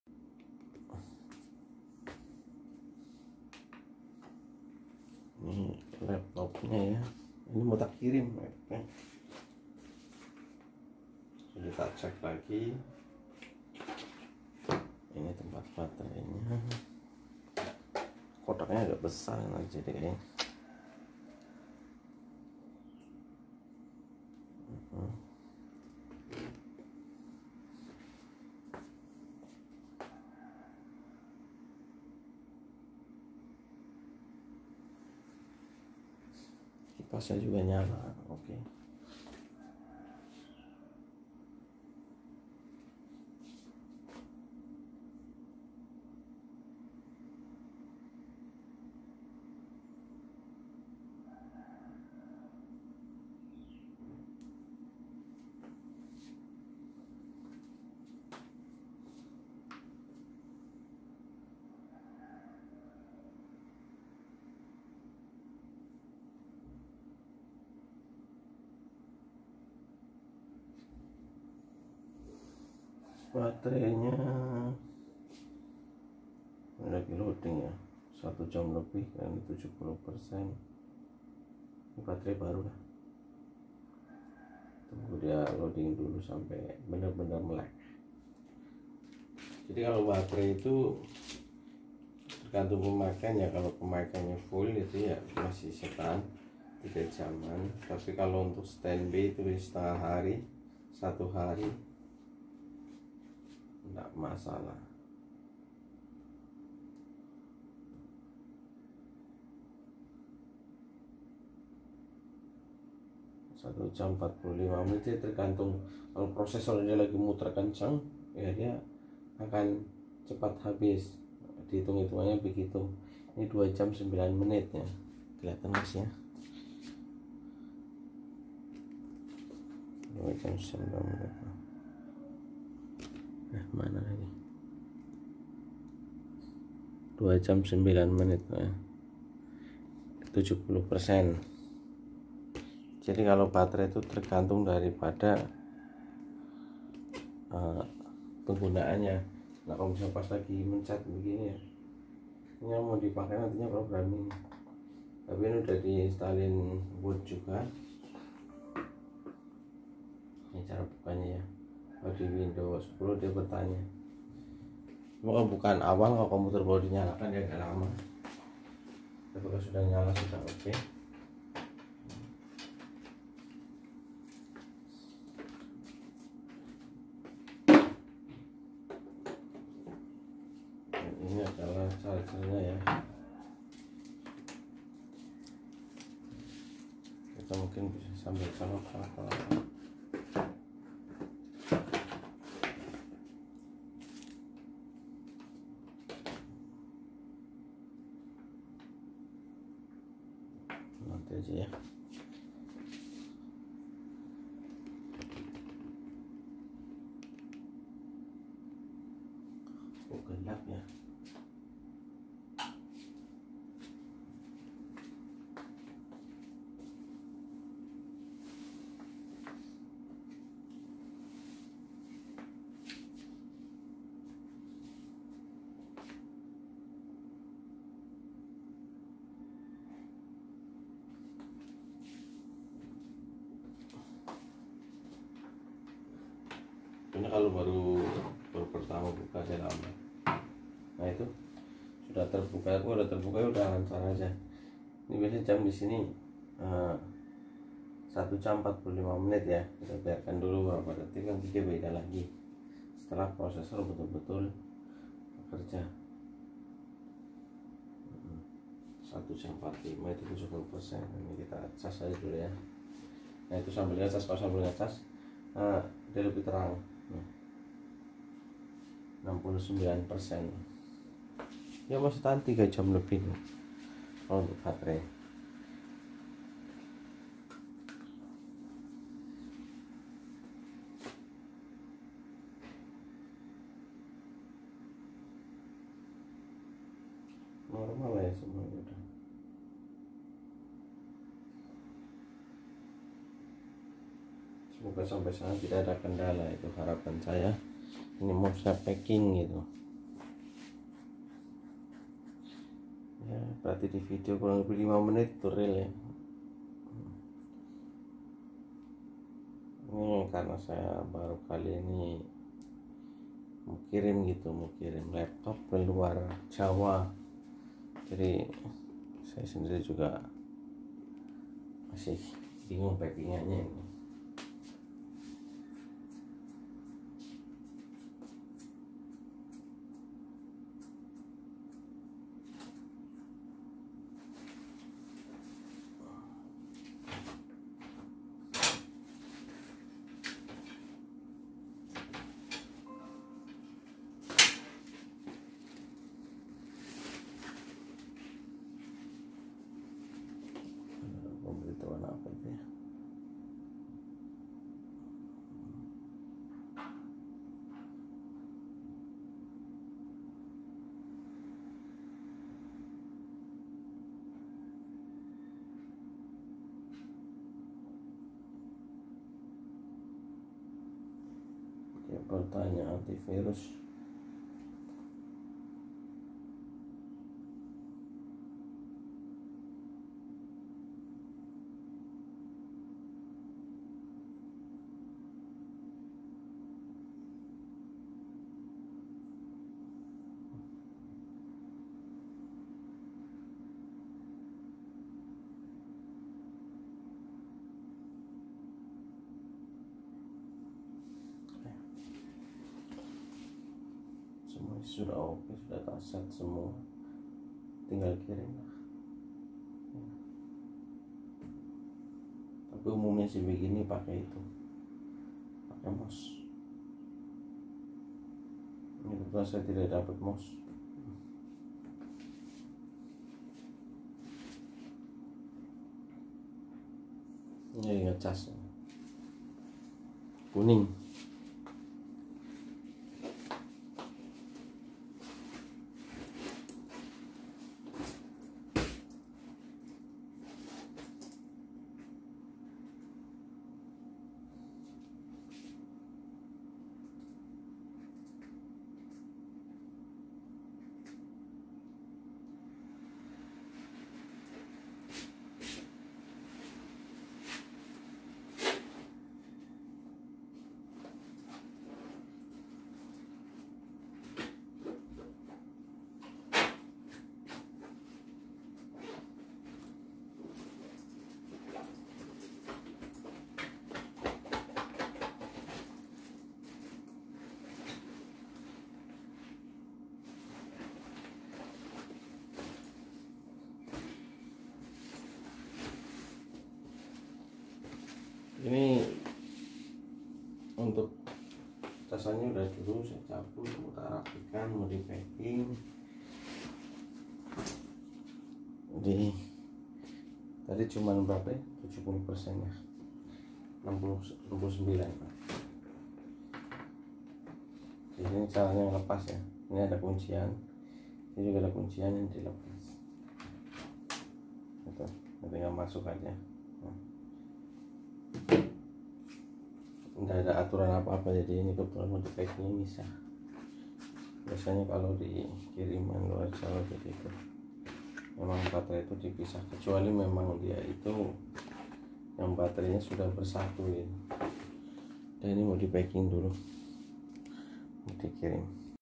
Laptop Toshiba Khusus program punching wilcom pesanan dari palopo sulawesi selatan
Laptop Toshiba Satellite Pro L650
Core i3 atau 3 CPU
Ram atau memeory 4GB ( 2x 2GB )
Hardisk 320 GB

Terinstal Windows dan aplikasi wilcom dokumentasi sebelum dikirim